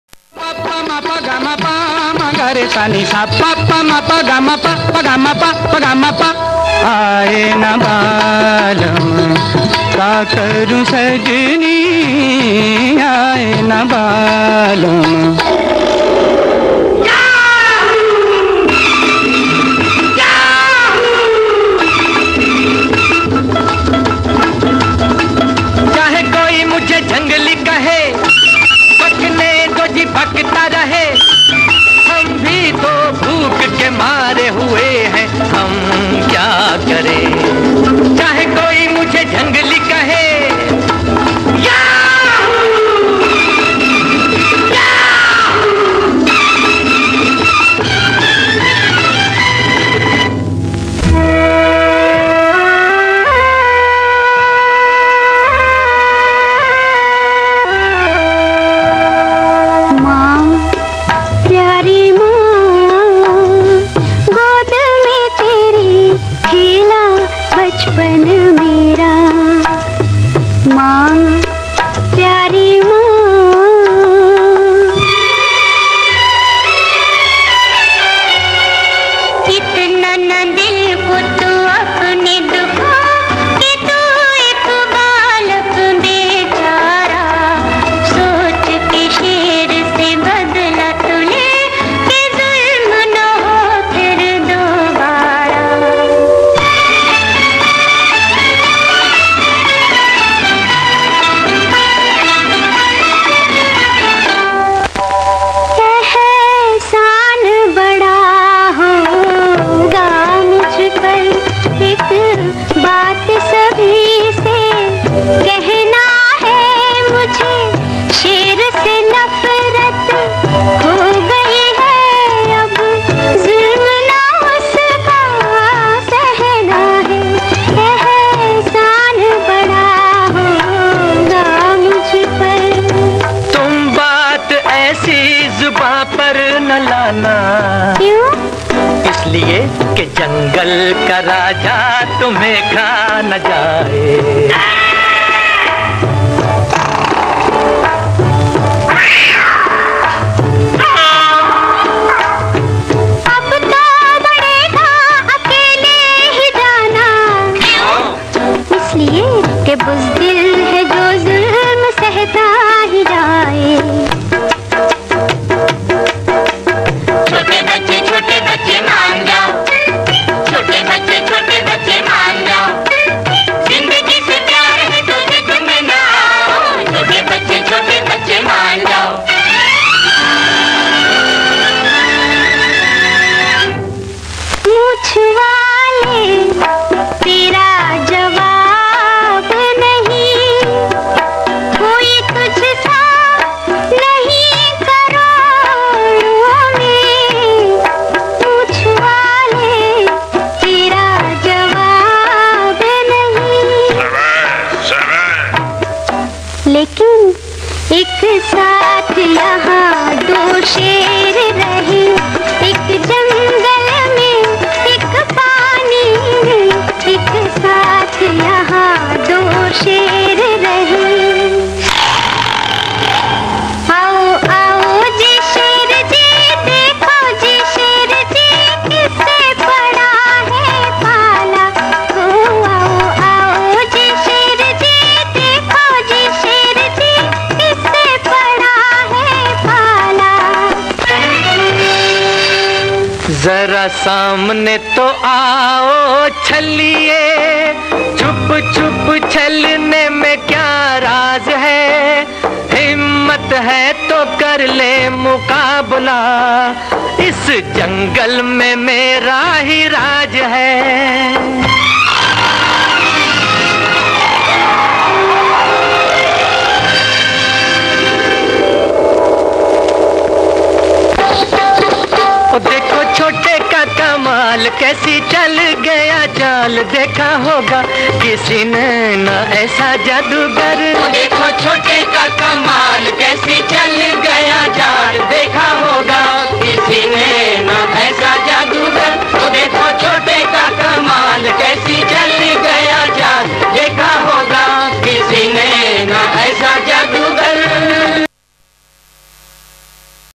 Naice Expression - 2013
This Dance specially performed on my school annual function.By my 4th to 7th standard students.I try to edit this dance something new experiment.Lets enjoy with this Dance.